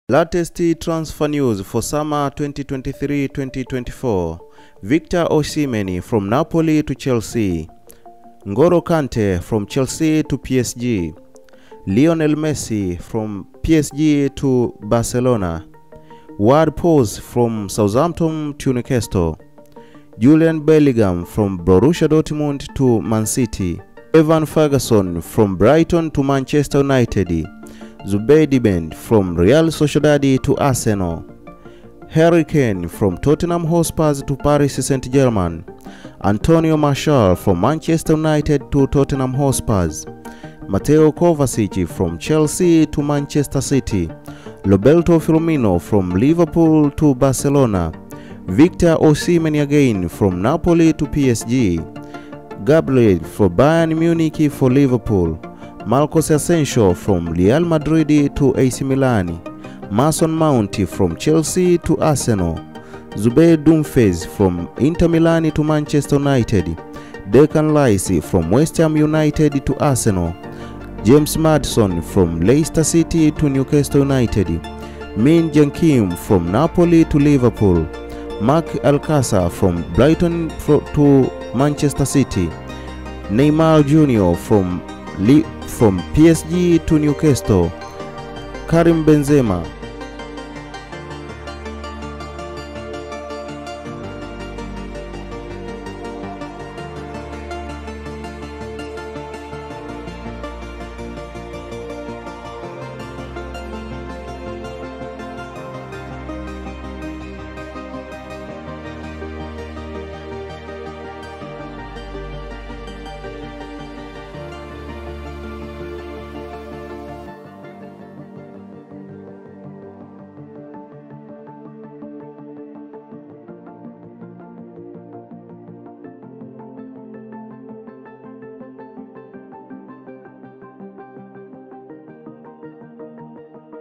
Latest Transfer Rumours 2023
List of transfer rumours 2023

Arsenal are monitoring Mason Mount's situation at Chelsea and could rival Liverpool for the 24-year-old England midfielder this summer. (Goal)

The Gunners have also sent scouts to assess Spain midfielder Martin Zubimendi, 24, and French defender Robin le Normand, 26, who both play for Real Sociedad. (Mail)

Paris St-Germain are preparing a £133m bid for Napoli and Nigeria striker Victor Osimhen and will move into pole position, ahead of Chelsea, for the 24-year-old. (Football Insider)

Mauricio Pochettino is the new front-runner for the Chelsea manager job after Julian Nagelsmann dropped out of contention. (Guardian)

Brazil striker Roberto Firmino, 31, has agreed to join Barcelona when his Liverpool contract expires this summer. (Mail)